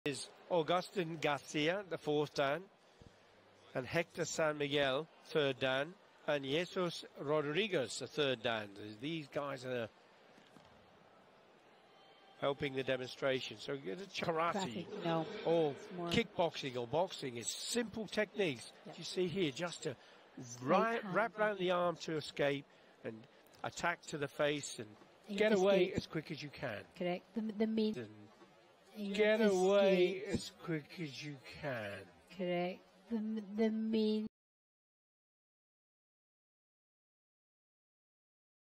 How to free yourself from a back grab | WORLD KARATE FEDERATION
Want to know how to easily free yourself from a back grab? Learn more with WKF's self-defense

Official YouTube channel of the World Karate Federation